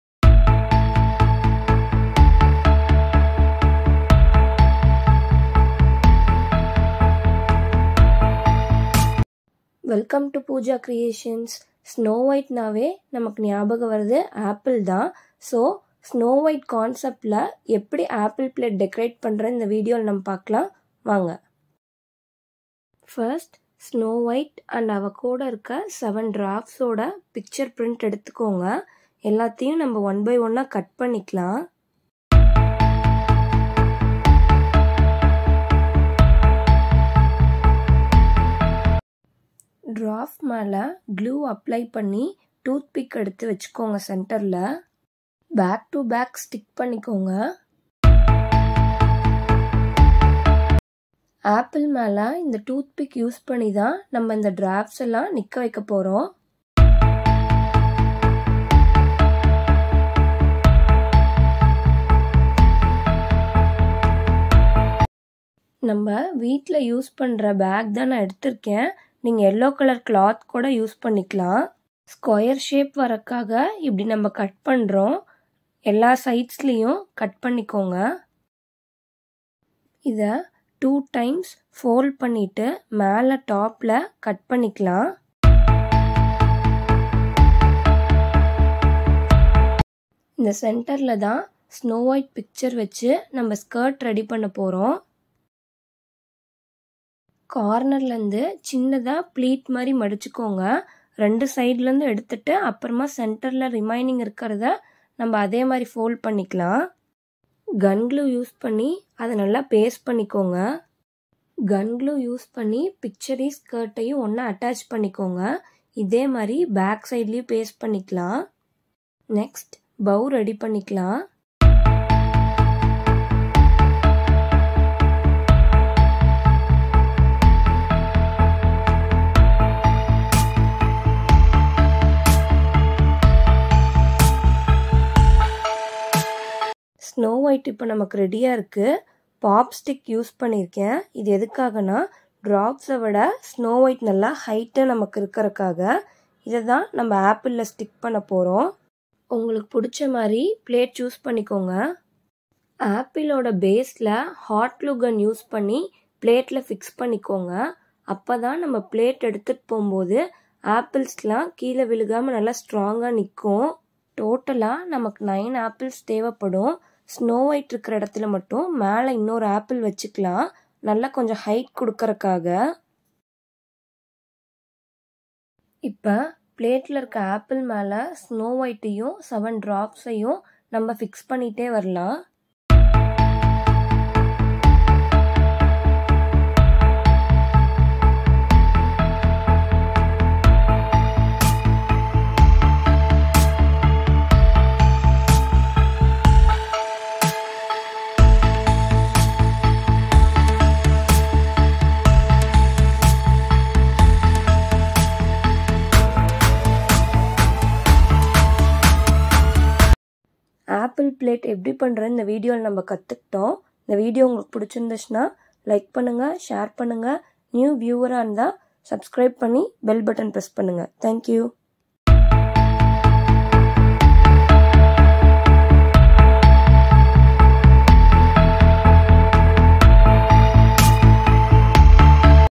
Plate decoration in tamil | Apple plate decoration | Fruits plate decoration
Hurry up! let's stop snow white from eating poisoned apple given by evil queen. Seven dwarfs (Bashful, Doc, Dopey, Grumpy, Happy, Sleepy, and Sneezy)are protecting her.
With snow white concept let's make apple plate decoration for engagement, baby shower, puberty, etc.,

Materials needed:
- Apple (9)
- Snow white & seven dwarfs pictures
- Toothpick
- Chopstick
- Fevi stick
- Hot gun glue
- Fiber plate (as you wish)